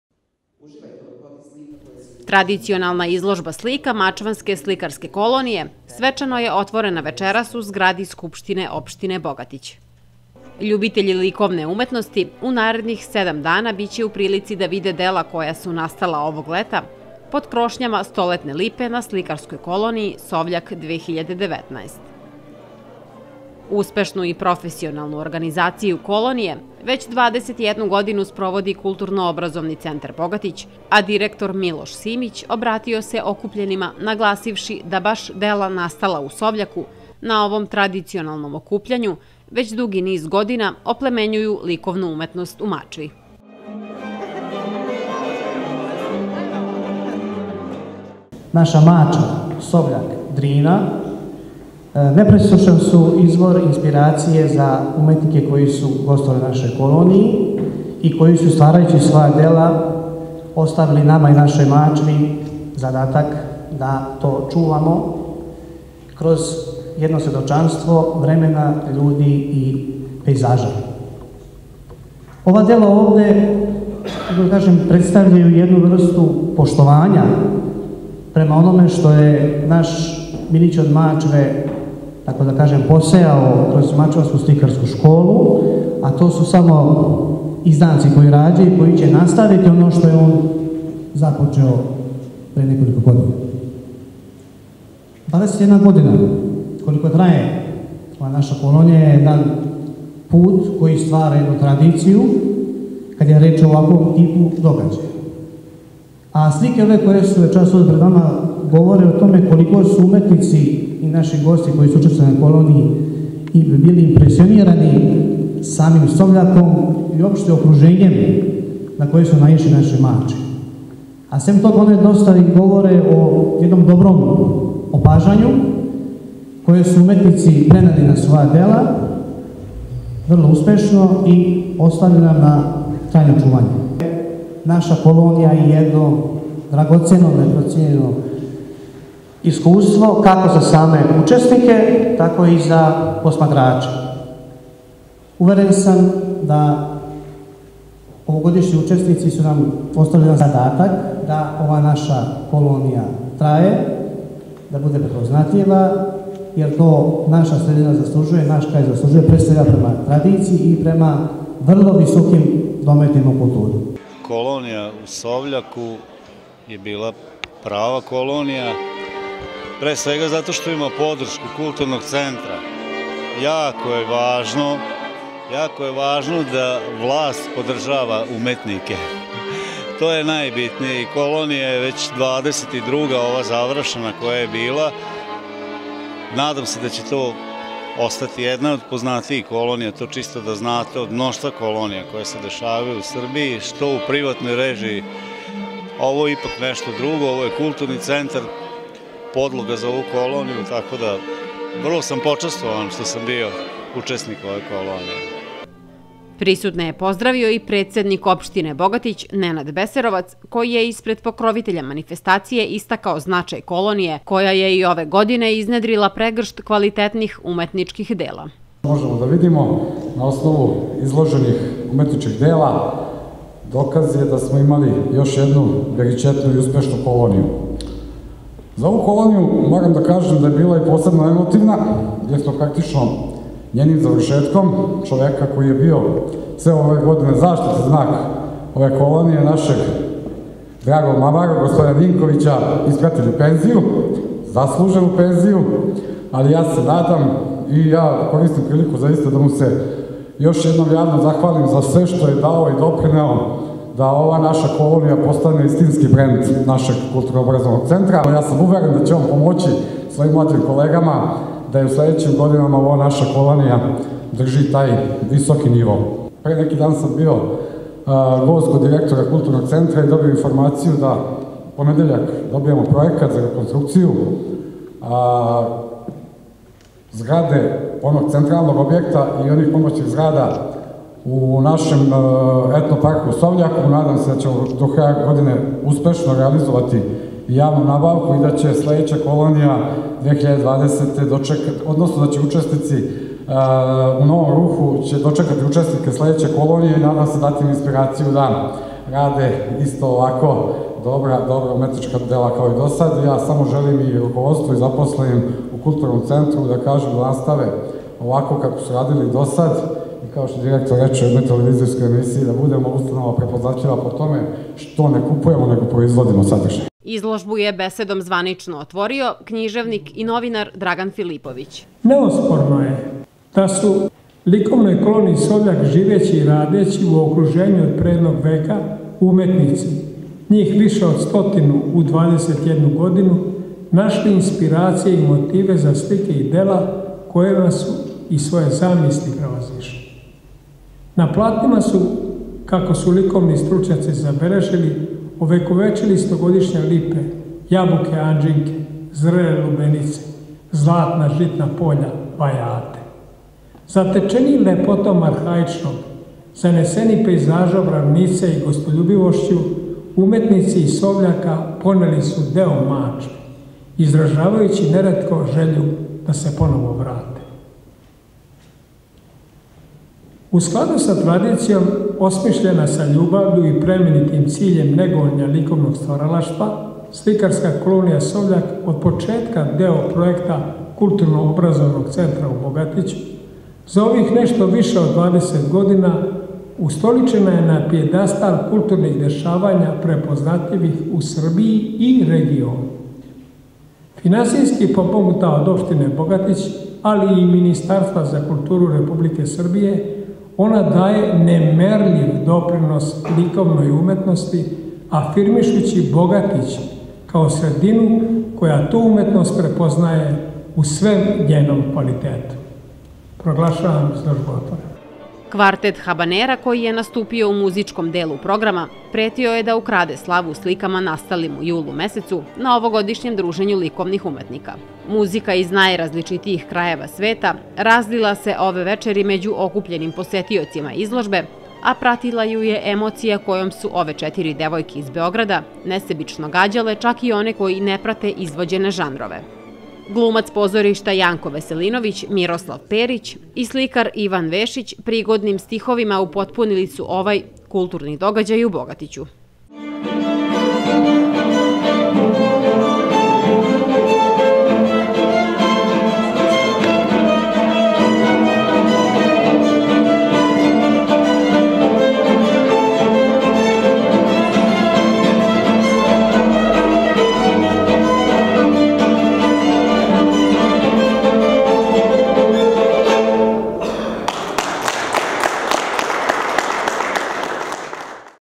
Отворена изложба слика Сликарске колоније „Совљак 2019“
Pripremili: Mira Kovačević, Marijana Kovačević
Proizvodnja: "Perfect" Bogatić, novembar 2019.